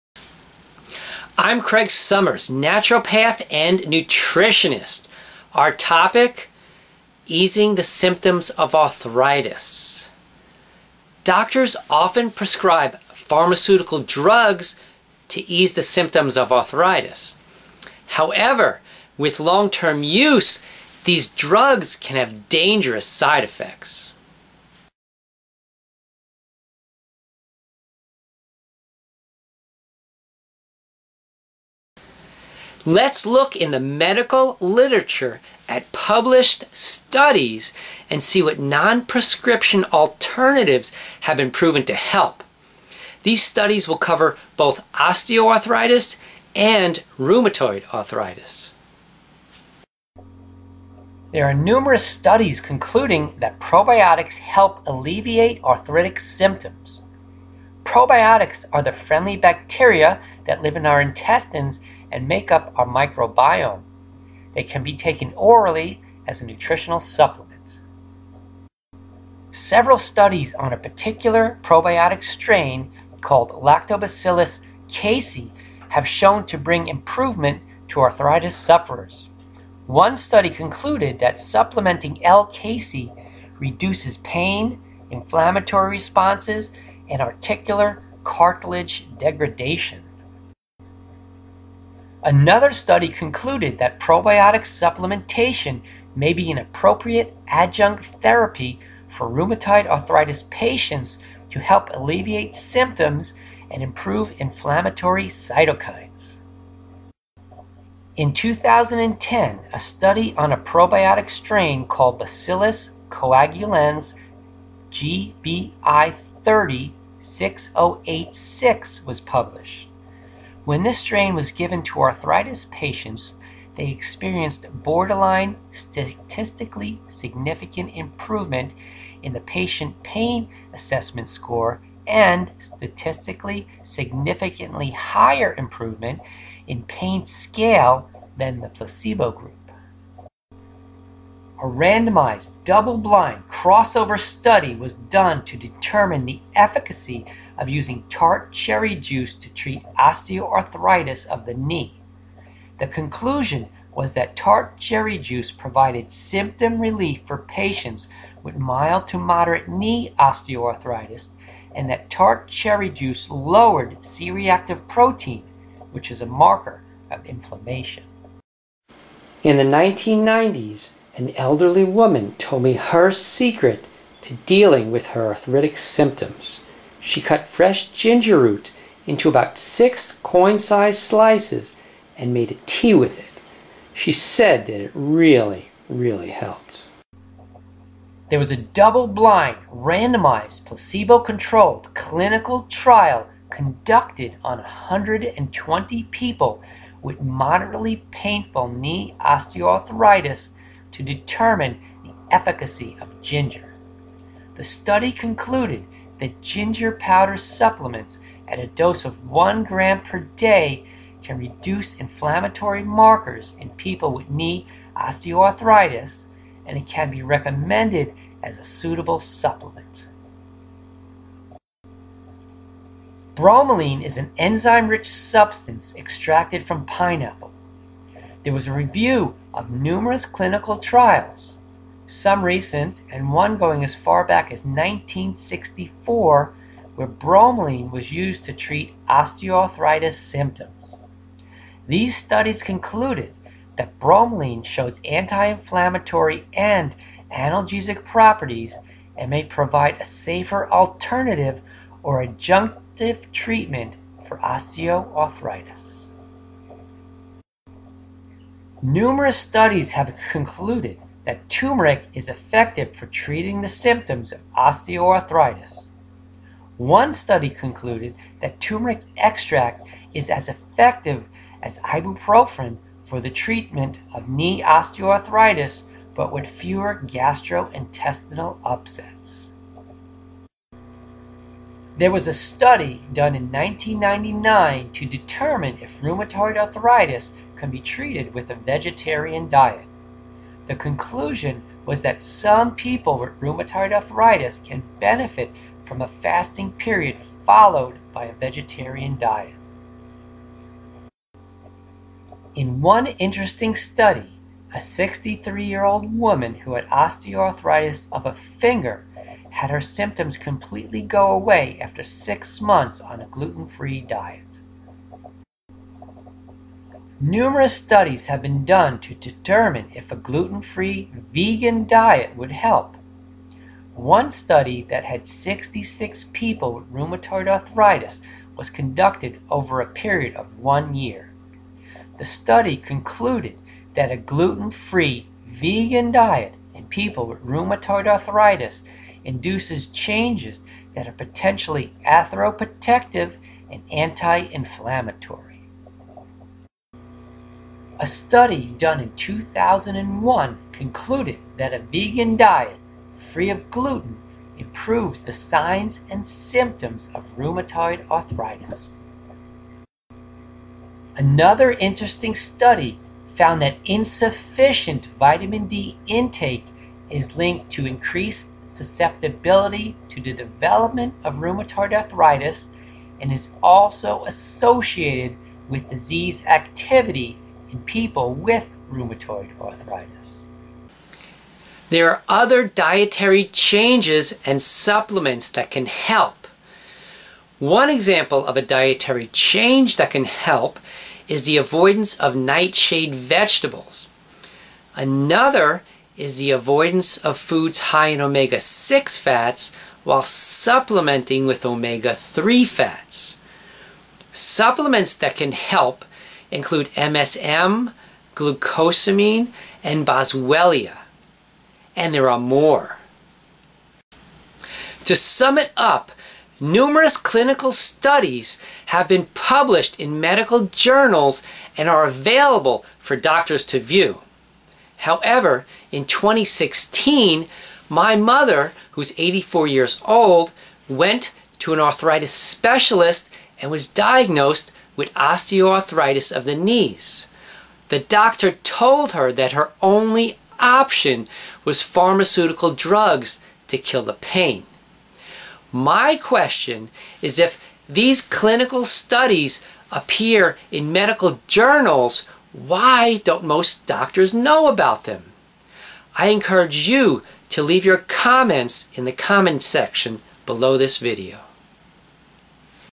Evidence for a Nondrug Approach to Ease Arthritis
A look in the medical literature at published studies about non-prescription natural alternatives that have been proven to help.